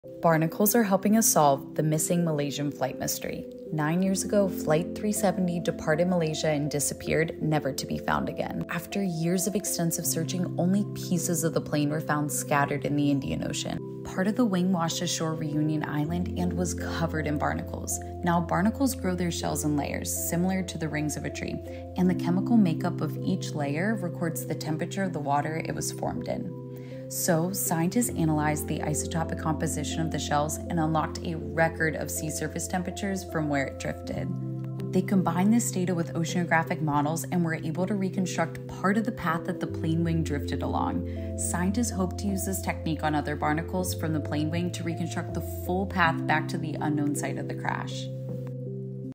Barnacle shells are a key to uncovering where missing flight MH370 went.
Scientists are using barnacle shells and oceanographic models to reconstruct where the missing Malaysian Airlines Flight MH370 crashed 9 years ago.

For more information, check out the publication describing this work titled, 'A Stable Isotope Sclerochronology-Based Forensic Method for Reconstructing Debris Drift Paths With Application to the MH370 Crash' by Al-Qattan et al. (2023) in AGU Advances.